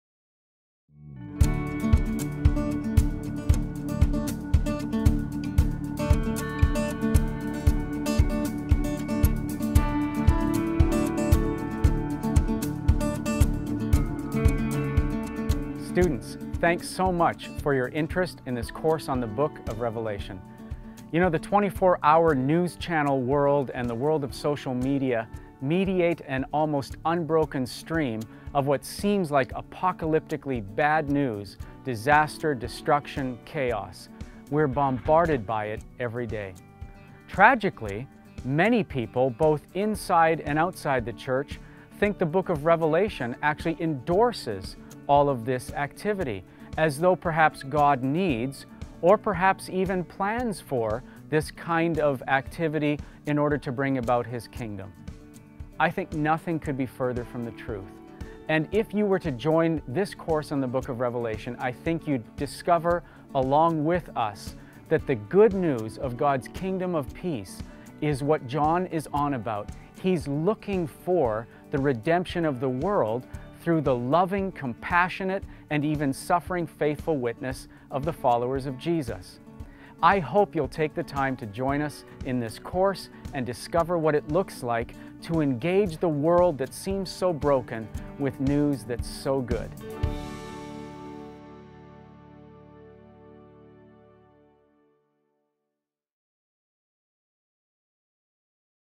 Course Trailer - Heroes, Dragons & Empires - Reading Revelation Then and Now
By far, the most misunderstood book in the entirety of Scripture is the book of Revelation. Many today see it as a portal into nothing but "end time" events. But Dr. Ron Herms helps us untangle the misconceptions and the misinterpretations by setting Revelation back into it's original context. To many students surprise, the original audience knew full well what John was communicating to them. Go to our website to learn more about 39 Class Teaching Course to gain the historical context, but also better understand the message of Revelation for us today.